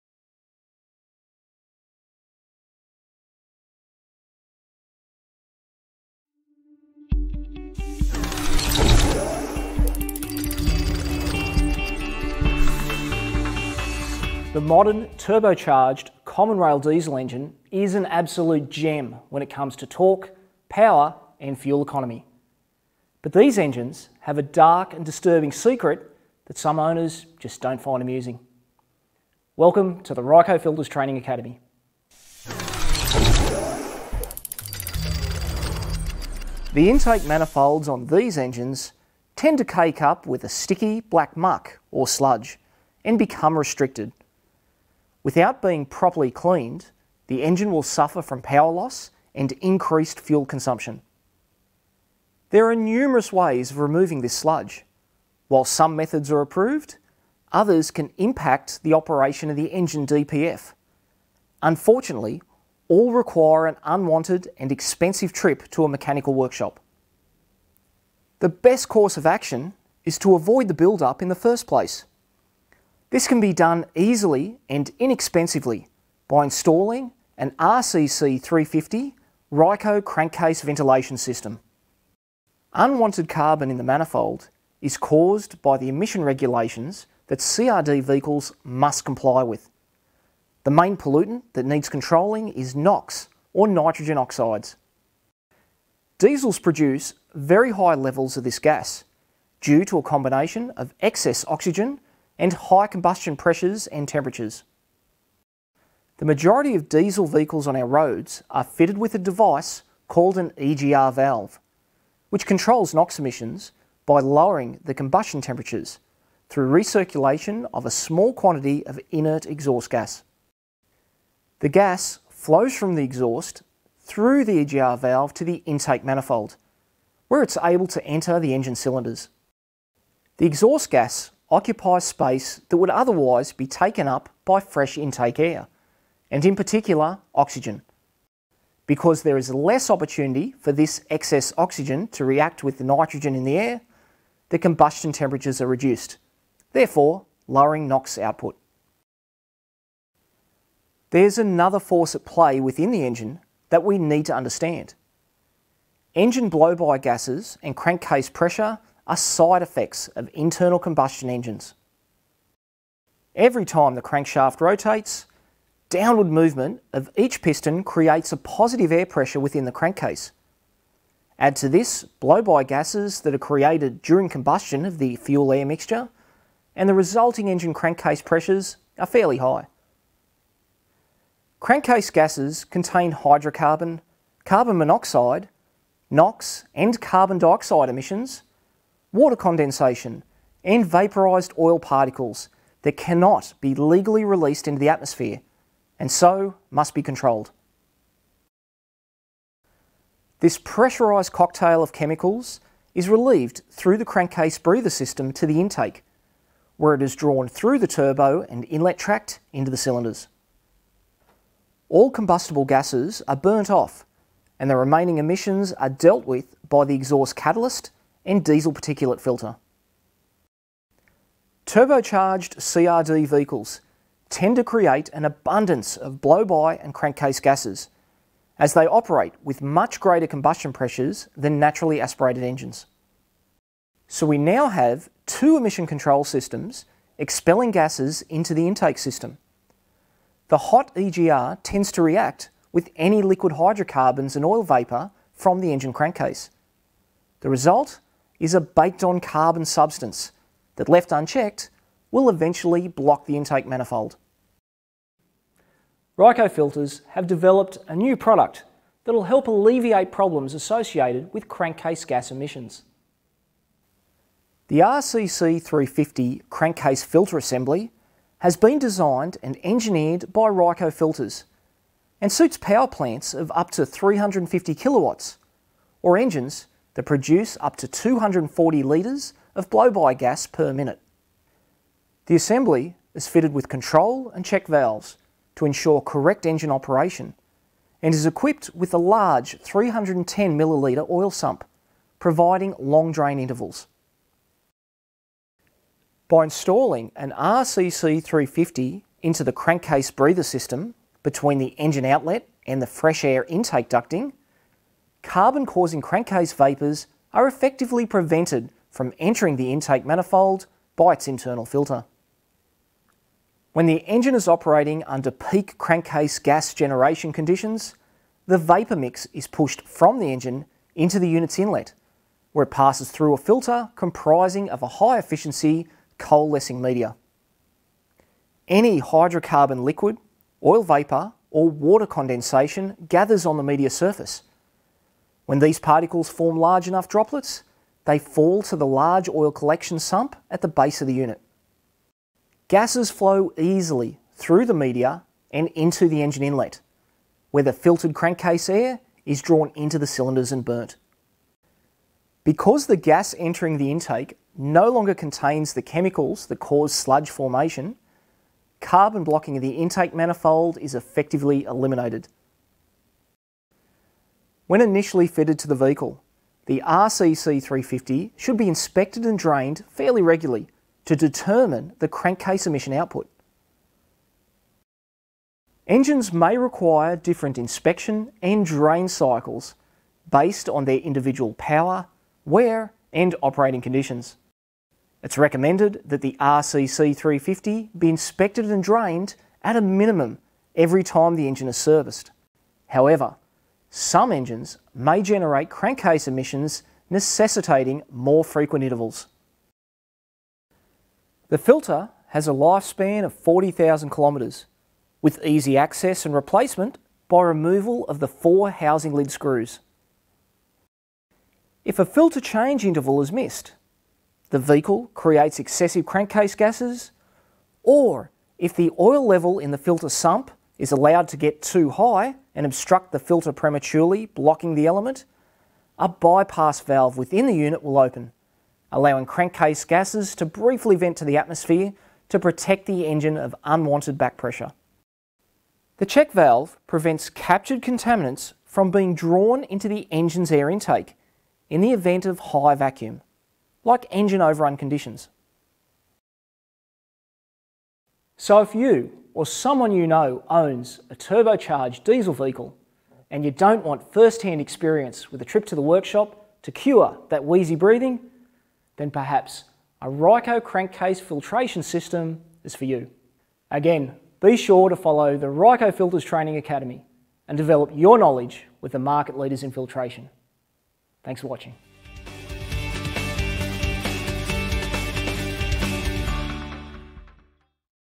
15 RYCO Crankcase Ventilation Explained
The Ryco Filters Crankcase Filtration System module, has been developed to introduce the learner to crankcase gasses, how they are produced, and the side effects of EGR and PCV control in both gasoline and diesel engines. By installing a Ryco Crankcase filtration system to affected engines, sludge and carbon build up is all but eliminated. The discussion includes: the operation and function of a PCV system, EGR system requirements, and side effects of emission control and installation and operation of a Ryco Crankcase Filtration System.
NOTE: The RCC350 catchcan mentioned in this video is now superseded to RCC351/RCC351K. See the Ryco catalogue for the latest listings.